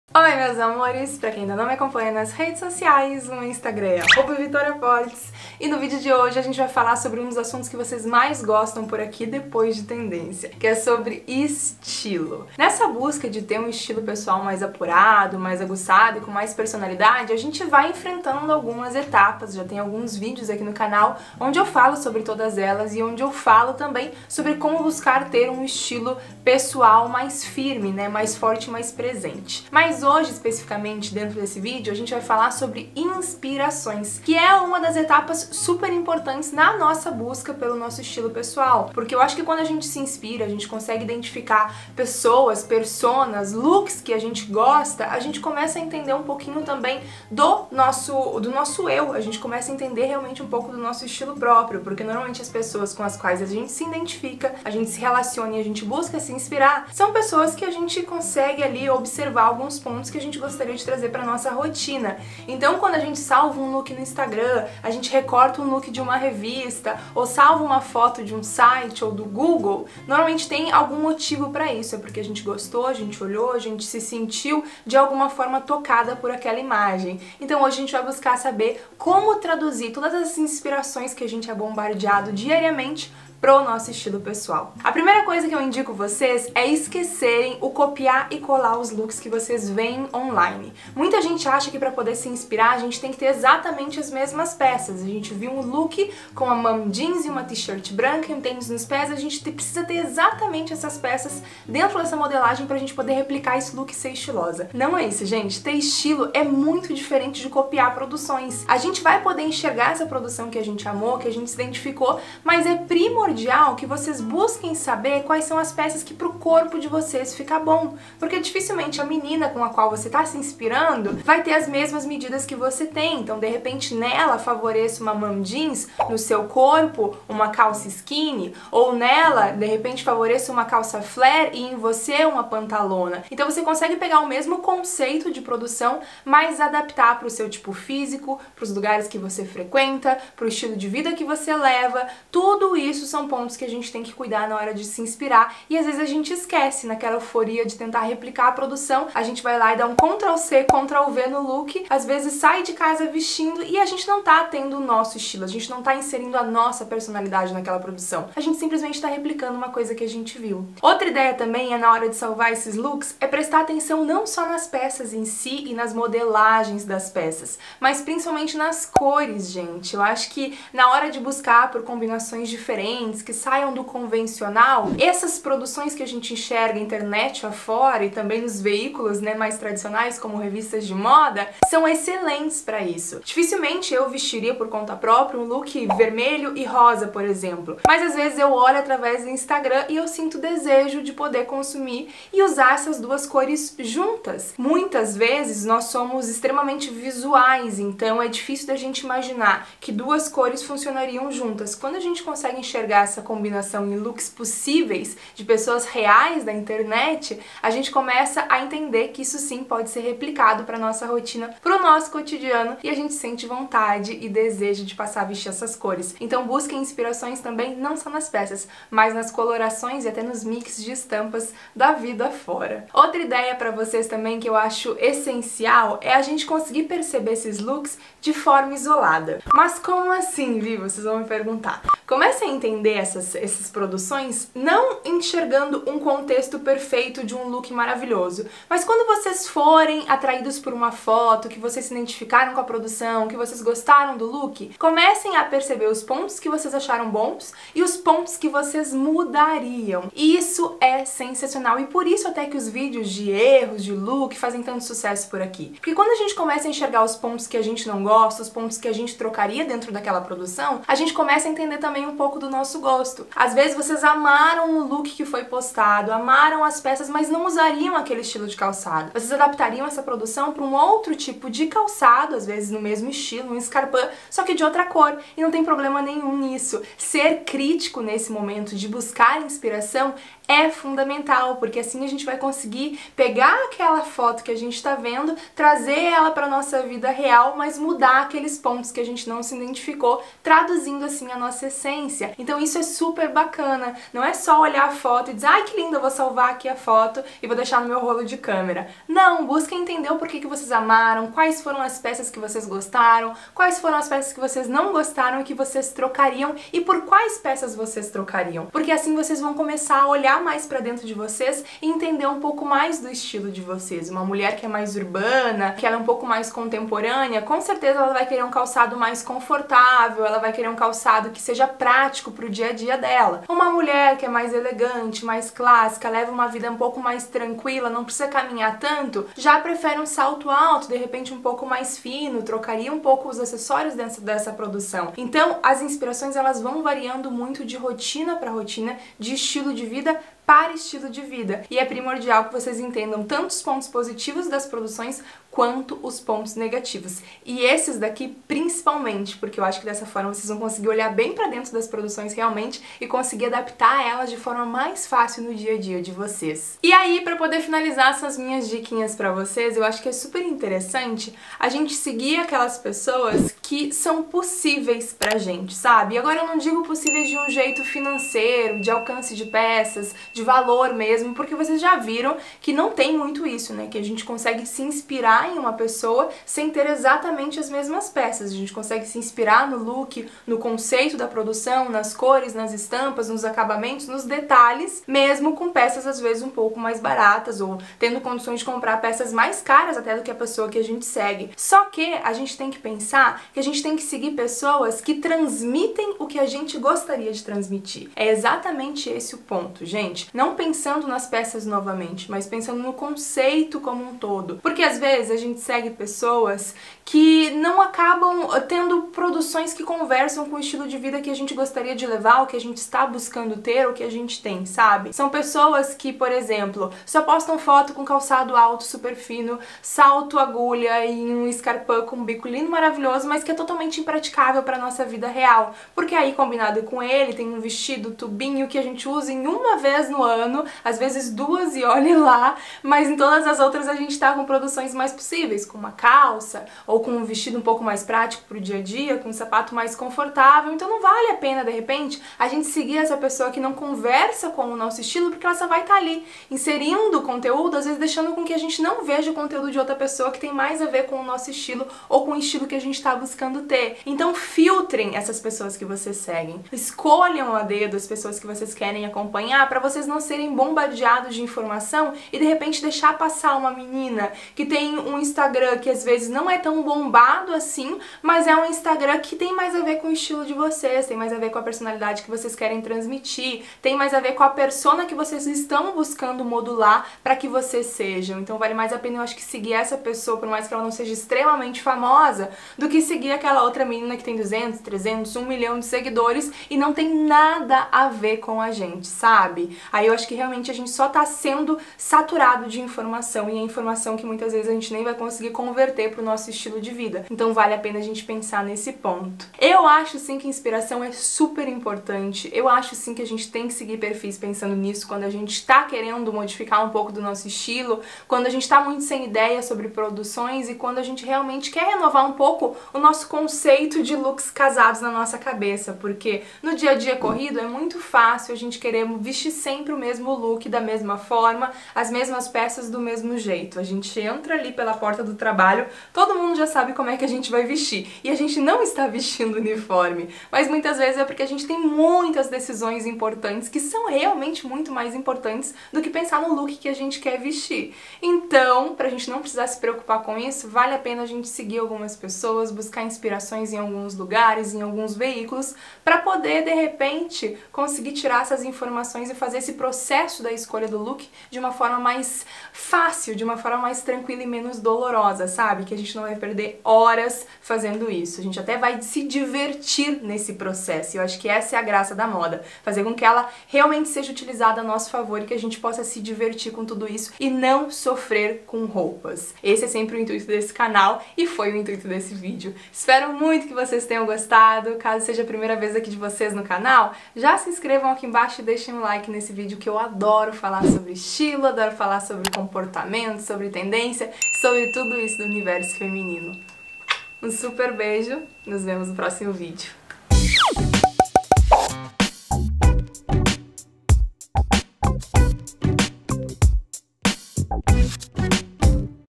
SAIBA COMO BUSCAR INSPIRAÇÃO NA MODA - Vitória Portes
Buscar inspiração é um dos processos na busca pelo estilo pessoal! No vídeo de hoje trago algumas dicas para aproveitar ao máximo as fotos que nos inspiram.